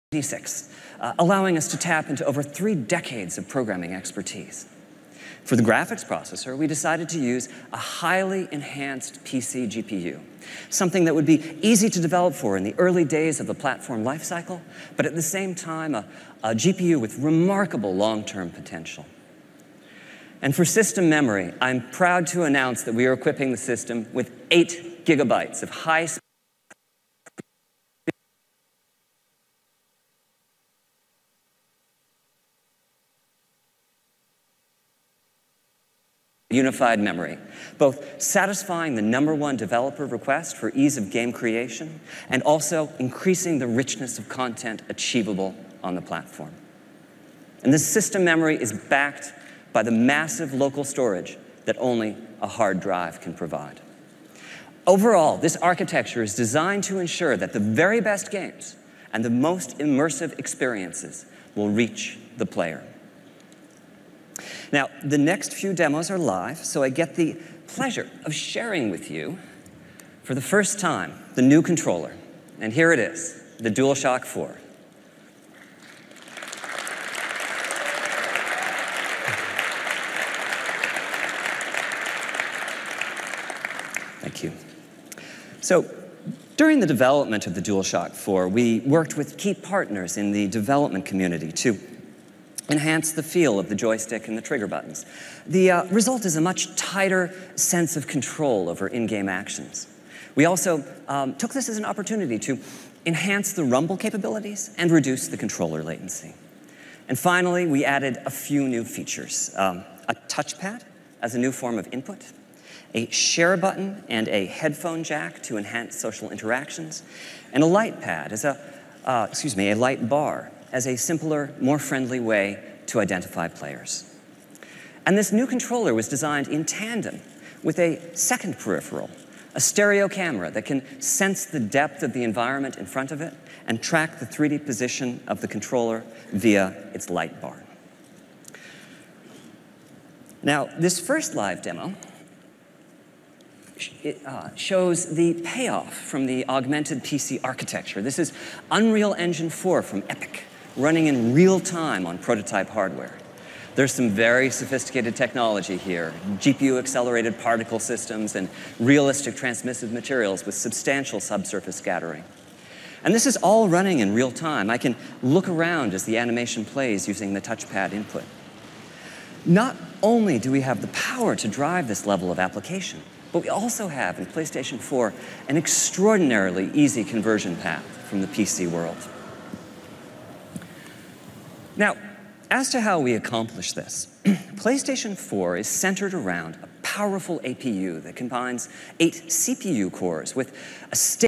PS4 - Playstation 4 Console Revealed - Sony Event 2/20/2013
We have a Minecraft server! Come discuss the PS4 with us! Server IP: mc.goldrealms.com

Sony finally revealed the Playstation 4 console at the Sony Event